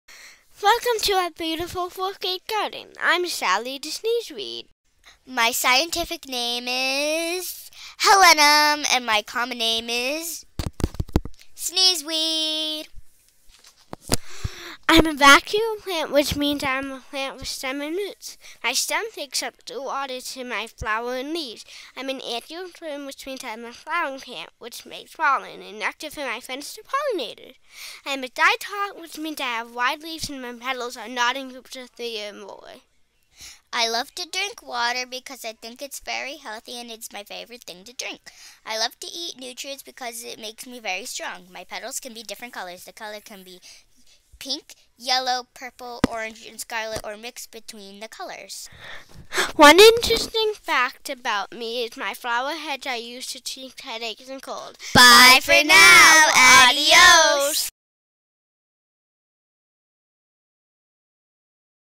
Meet the Sneezeweed
This video is about the sneezeweed plant in the Briggs Elementary School science garden.